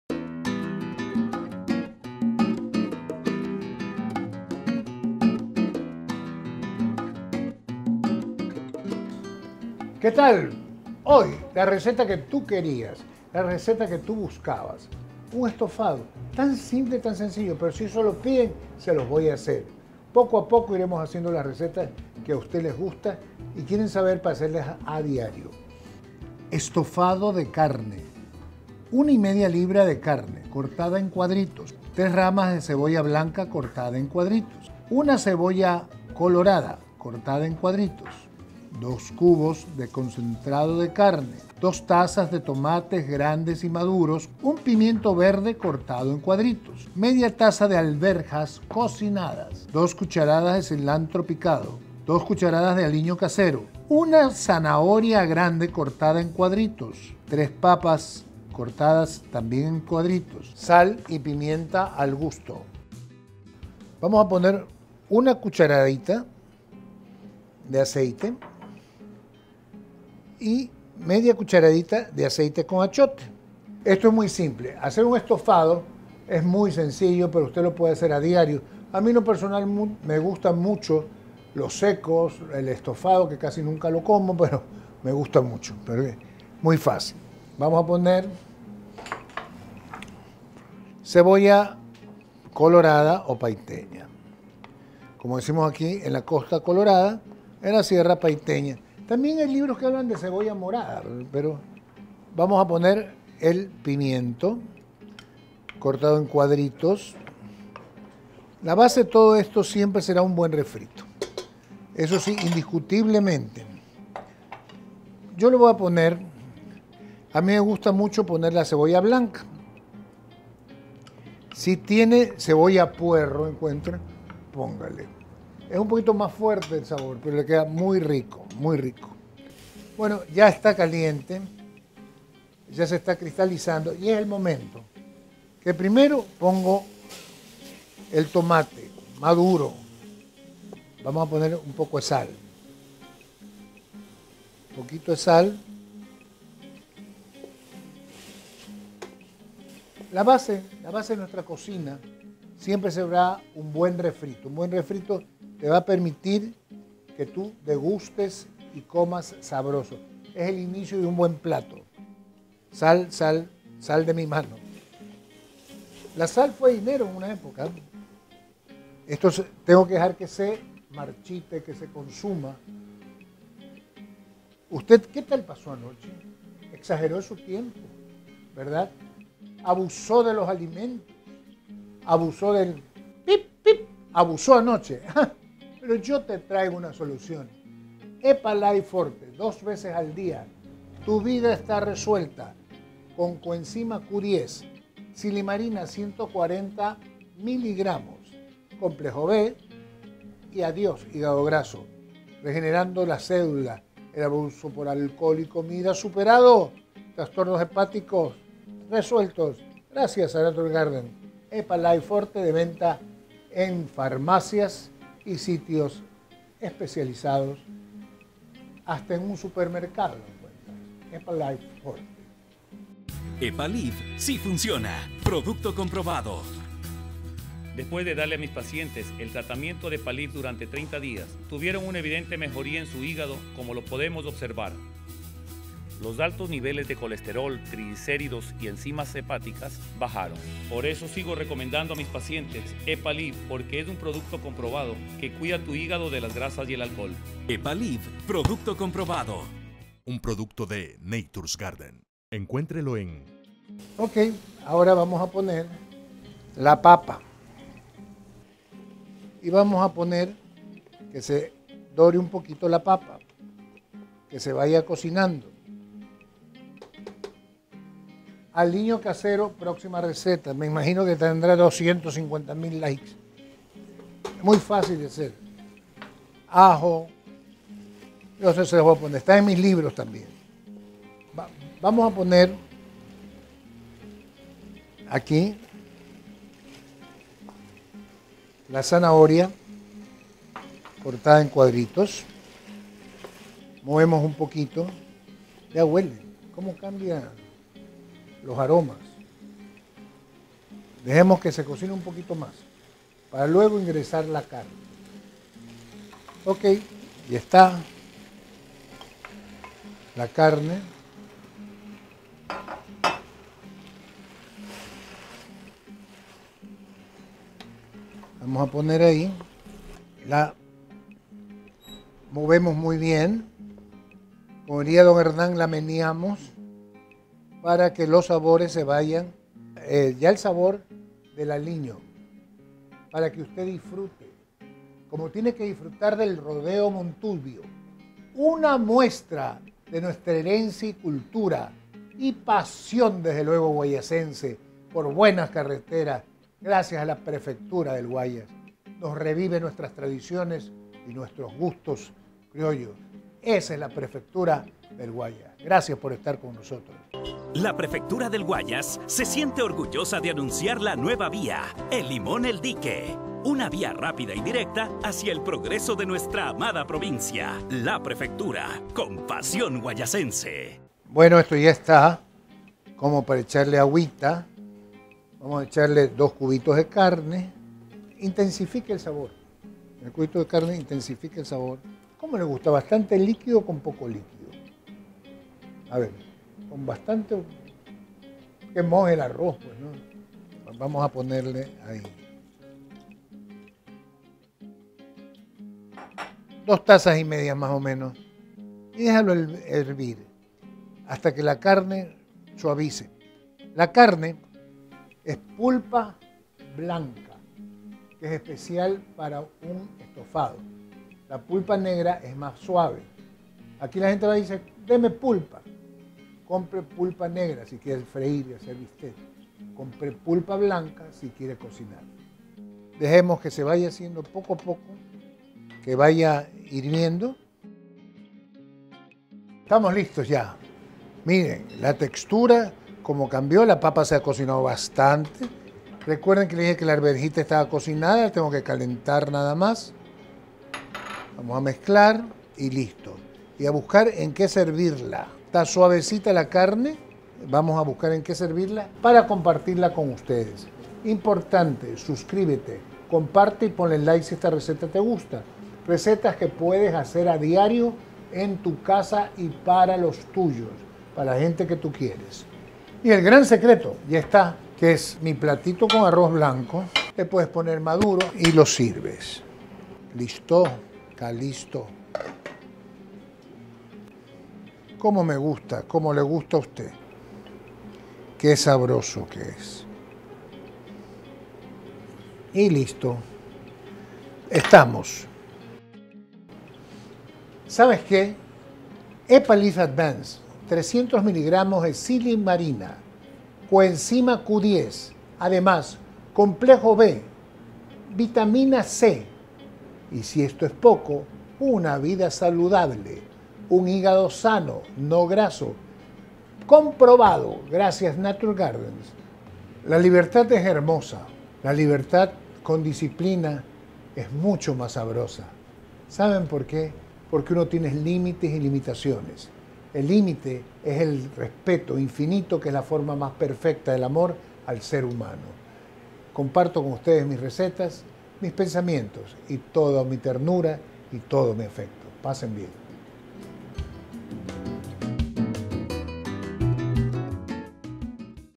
Molinari tv Cocina Fácil | Estofado de Carne
Económico, rico, fácil de preparar y una  excelente fuente de energía, con este plato típico de nuestra gastronomía sorprende a tu familia y a tu seres queridos, que lo disfruten!